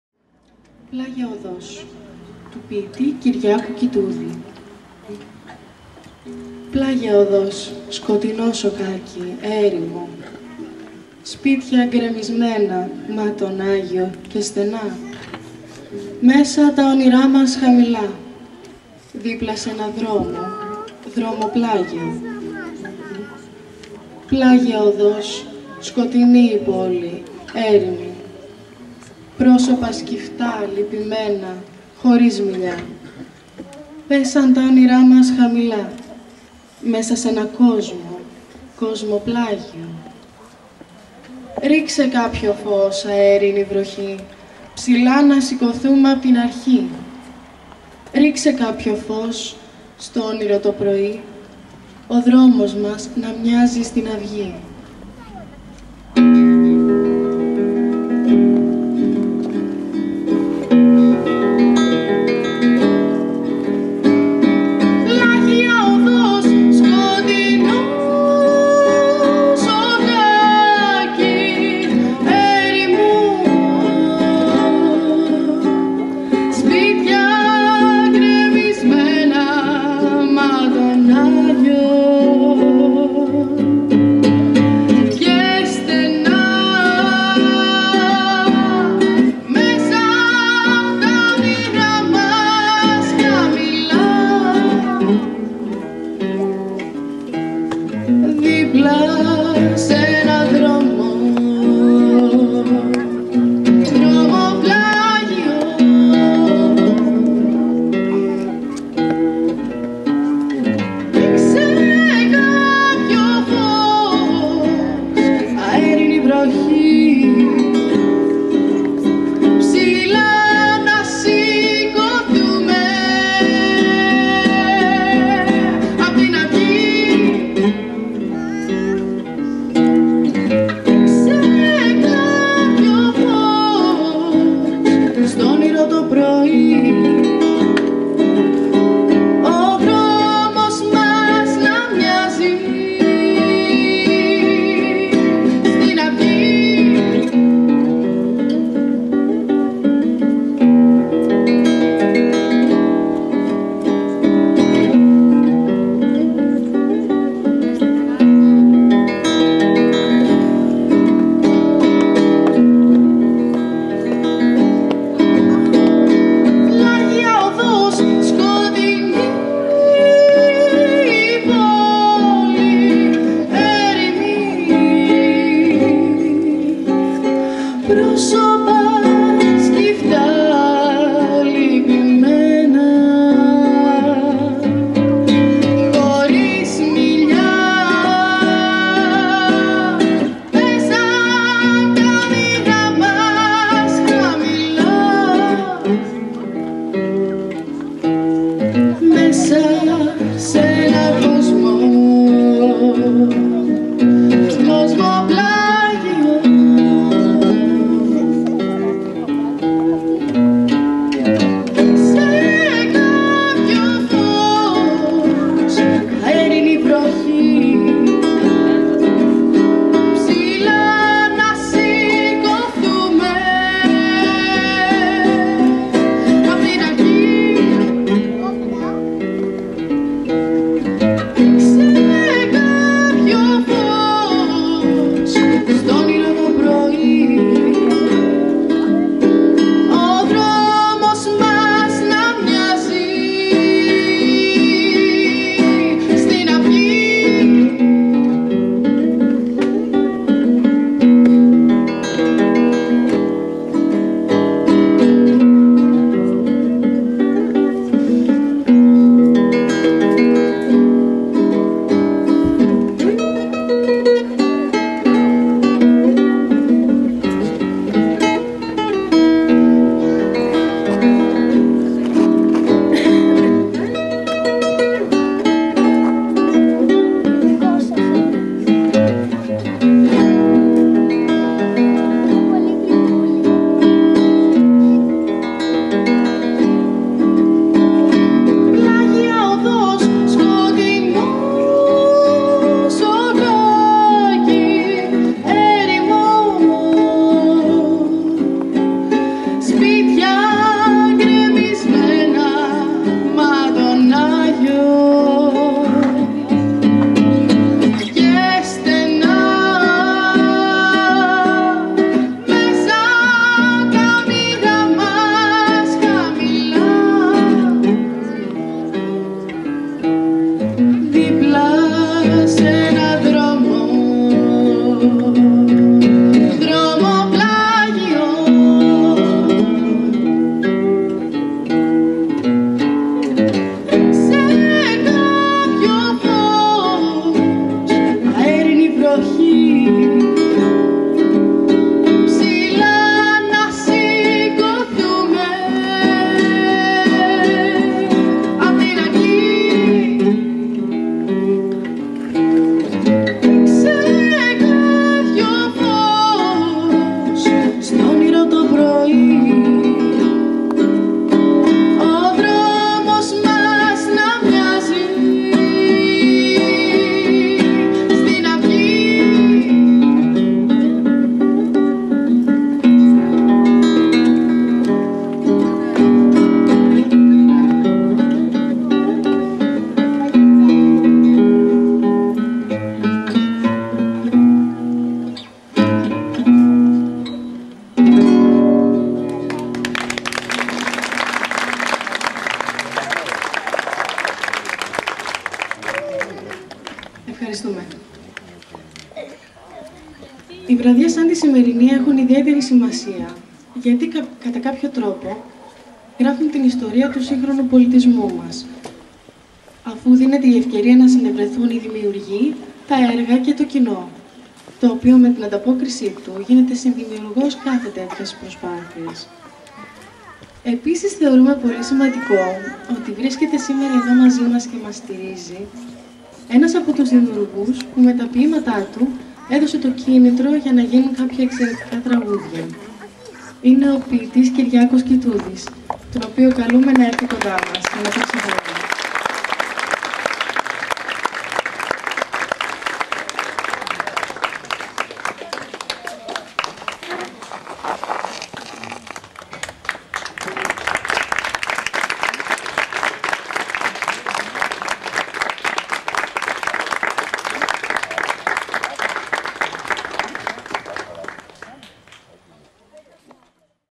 Ποιήματα σε νότες -Αγ.Γεώργιος Νηλείας-Πλάγια οδός
Κυριακή 2.08.09 Αγιος Γεώργιος Νηλείας 23 χλμ απο το Βόλο, 700 μέτρα υψόμετρο με φόντο όλο τον Παγασητικό κόλπο, στο προαύλιο χώρο του ομώνυμου Ιερού Ναού.
Πανέμορφη βραδιά με ένα δροσερό αεράκι να κατεβαίνει απο την ρεματιά. με το φεγγάρι πάνω απο το κόλπο να καθρεφτίζεται απολαύσαμε τραγούδια σε διαφορετικά στύλ απο τον συνθέτη και κιθαρίστα Γιώργο Φουντούλη με την ζεστή φωνή της Νικολλέτα Γιαννουτάκη, σε βραβευμένα ποιήματα και στίχους σύγχρονων ποιητών ¨Μάρως Βλάχάκη, Ανδρομάχης Διαμαντοπούλου,Κυριάκου Κυτούδη,Χάρη Μελιτά,Γιώργου Χατζηπολίτη.
Η ζεστή φωνή της Νικολλέτας δένει υπέροχα με τη μουσική του Γιώργου Φουντούλη που την συνοδεύει με την κιθάρα του με ενα μοναδικό τρόπο.